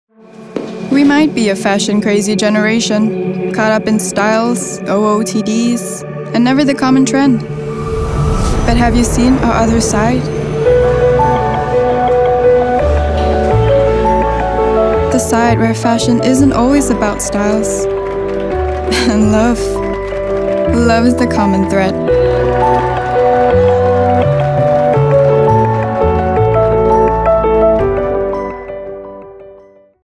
The Other Side of Singapore Fashion Runway
Fashion isn’t always about trends or styles. Beyond designing and stitching, youth designers of Singapore Fashion Runway are giving meaning to the art by using it to empower members of society. See the other side of Singapore Fashion Runway.